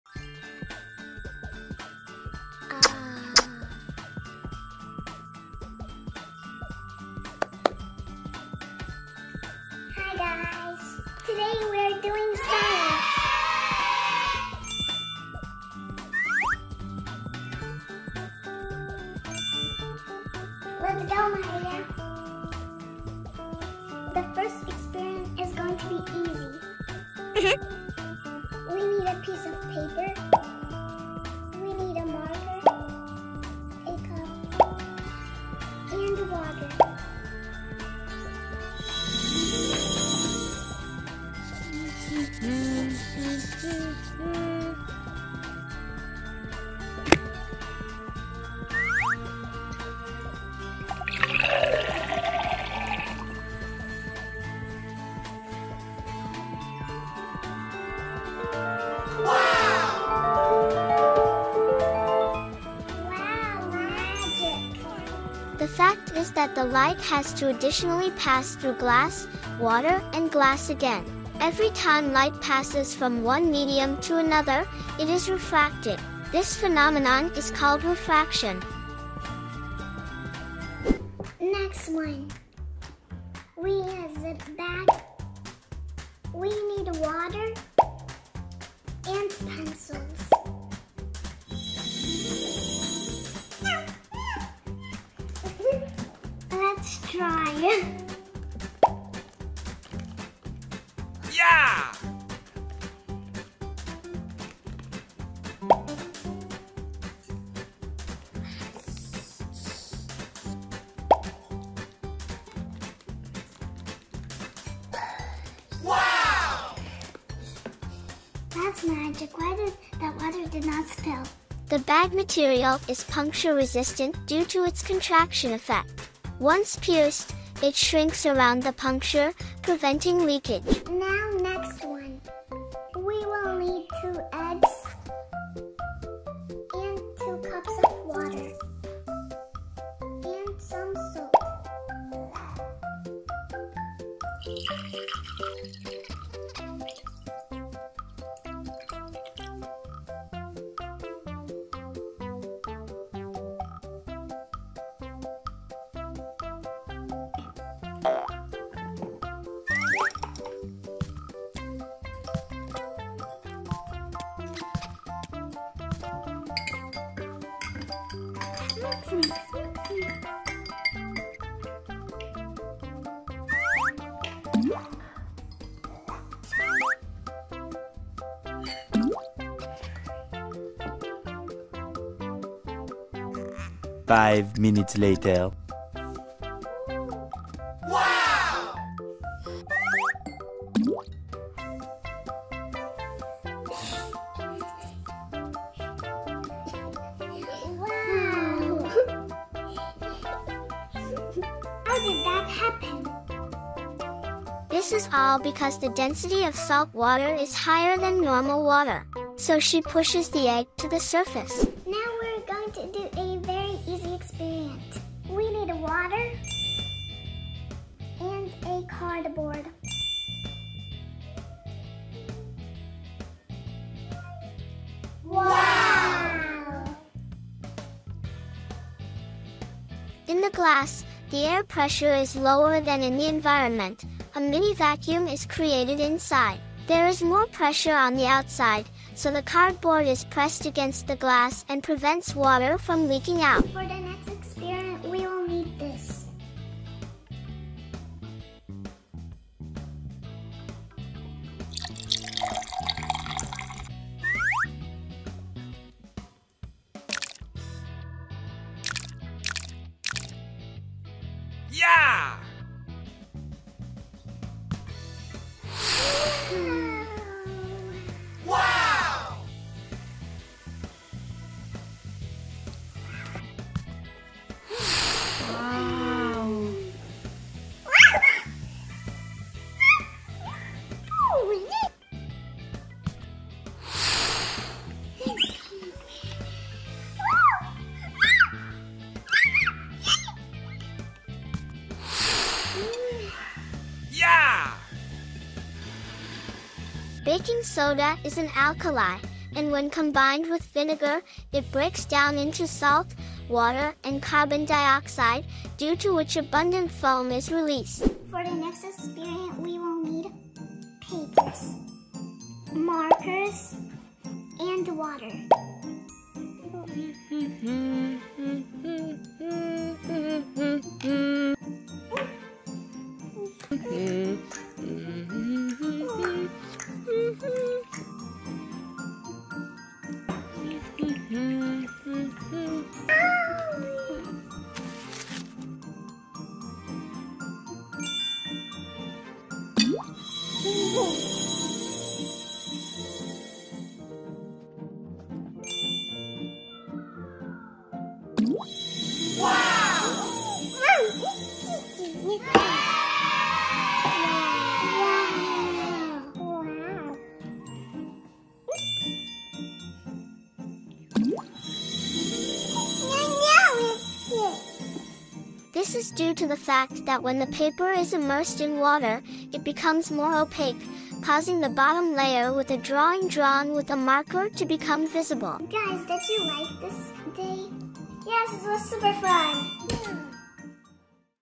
Girls doing experiments at home.
The girls decided to conduct experiments at home. They do experiments with eggs, blow up a colorful volcano and do experiments with water and colored pencils!